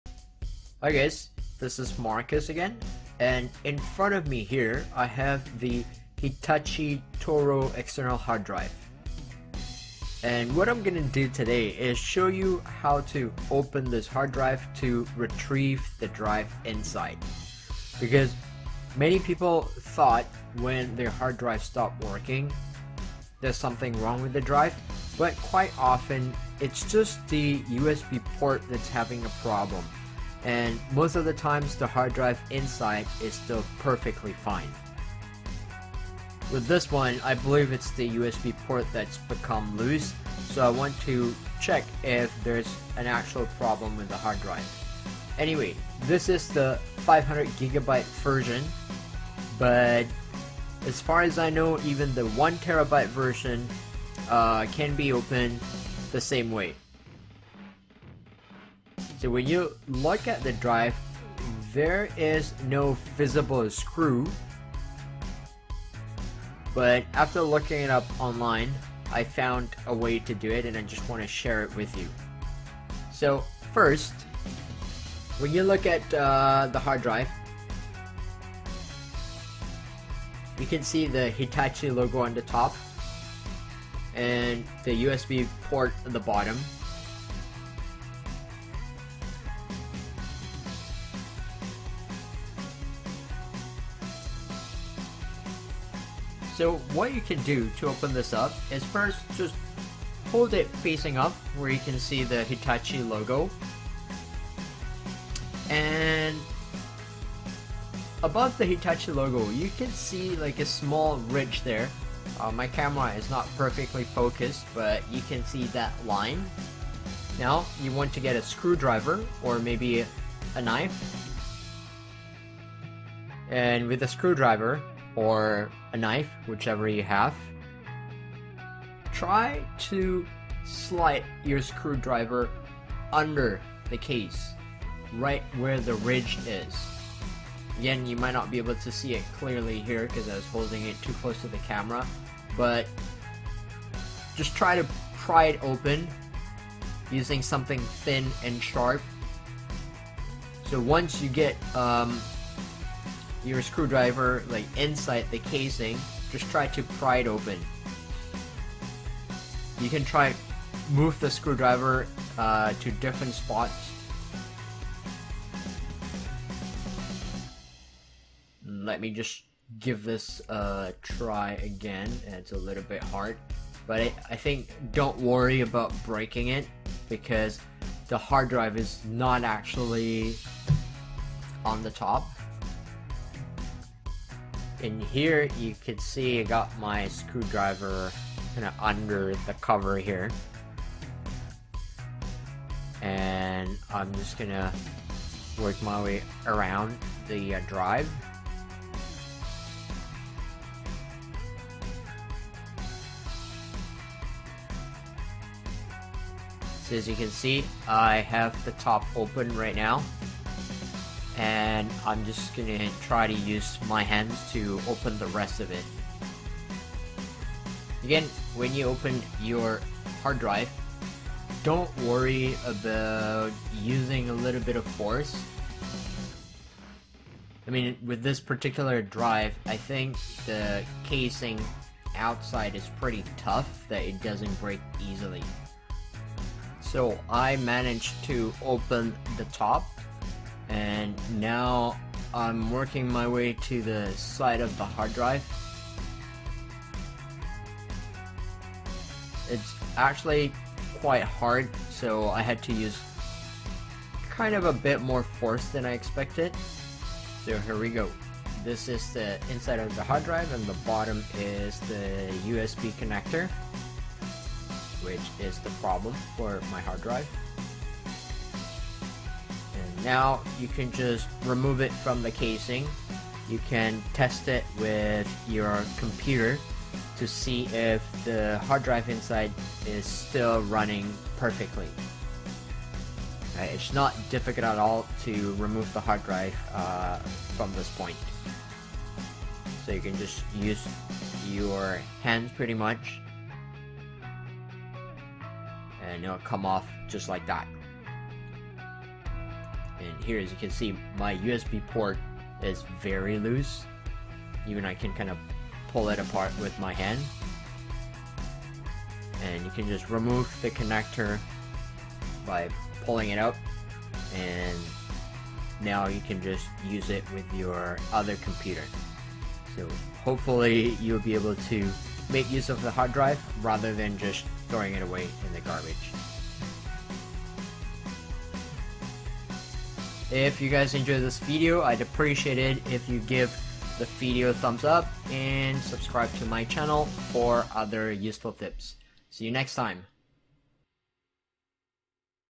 Don't throw away your external drive yet! How to open Hitachi Touro
Sometimes, your external drive is still running flawlessly but it has a problem with the SATA-USB port. In that case, you can remove your hard drive and use it with your laptop, desktop or maybe your XBOX too.